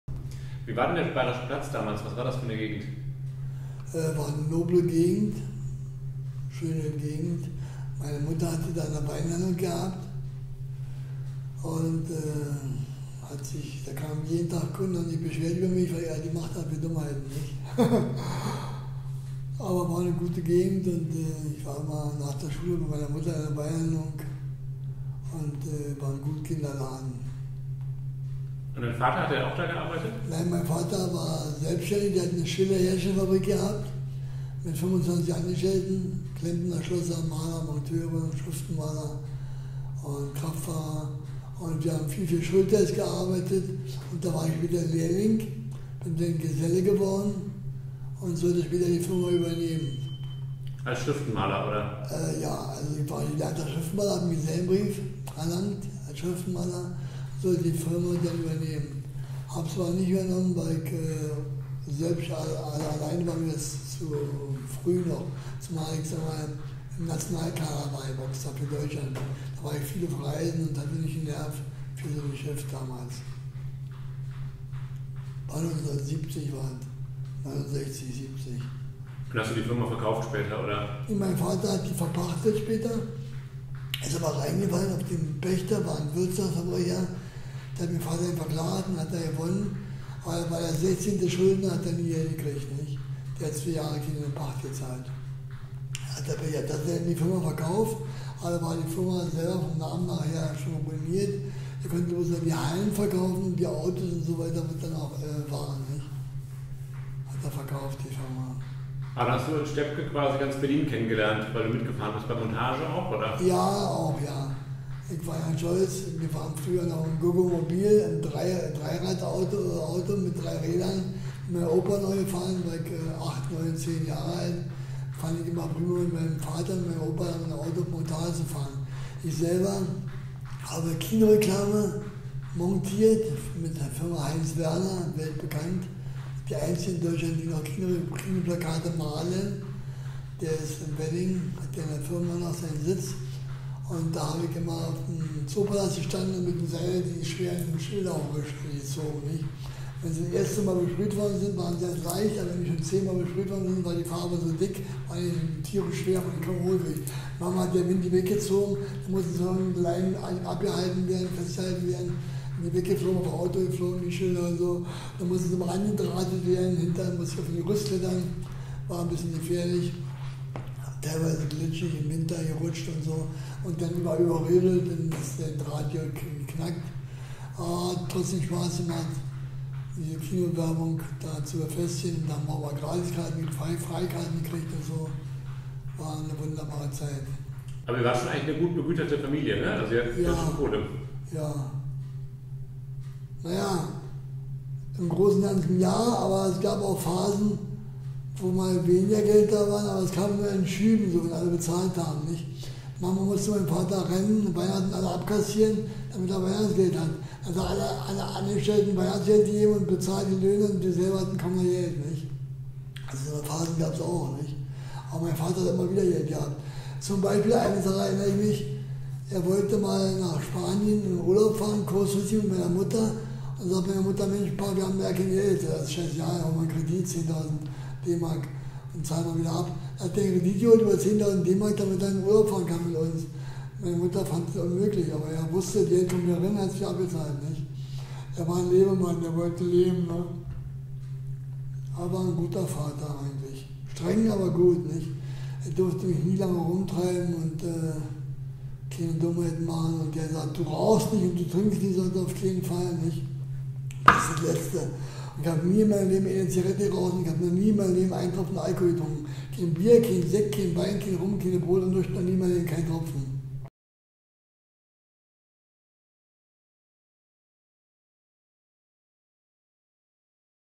Berliner Kindheit (*1947) - 2 "Schriftenmaler"
Der Tod seines älteren Bruders ist für Bernd Jacobitz (*1947) der Beginn seiner Laufbahn als Boxer. Er erzählt wie er dem bewunderten Bruder nacheifert und seinem Vater den Bruder ersetzen will. Es folgen erfolgreiche Jahre als Profi-Boxer, die über die Berliner Meisterschaft ins Nationalkader führen, bis ihn eine Verletzung aus der Bahn wirft. Boxer der er ist, steht Bernd Jacobitz aber auch nach weiteren Schicksalsschlägen immer wieder auf. Das Buch zum Projekt "Berliner Kindheiten" ist bei Transit erschienen